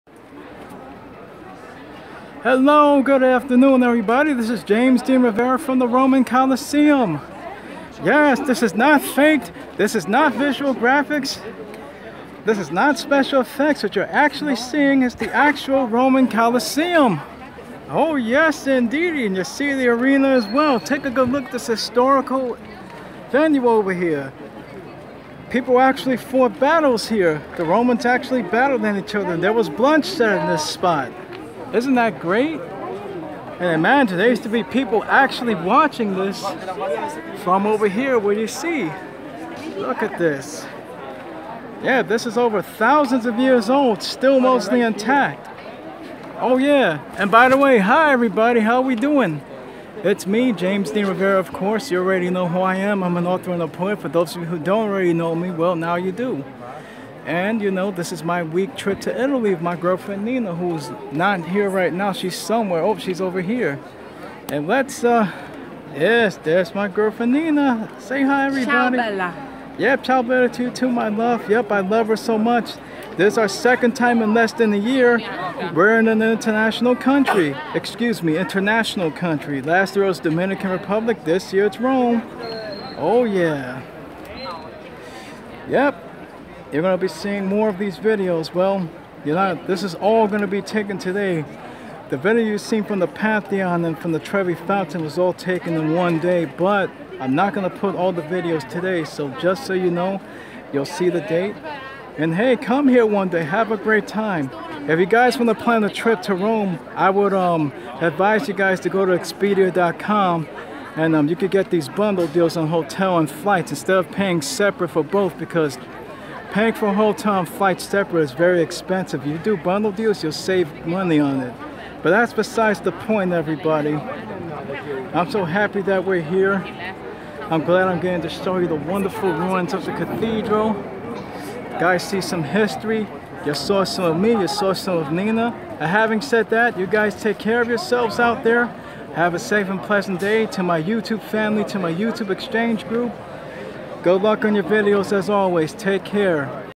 The Italia Series Part One: At The Rome Coliseum
Nina and I visiting the Rome Coliseum on our second day in Rome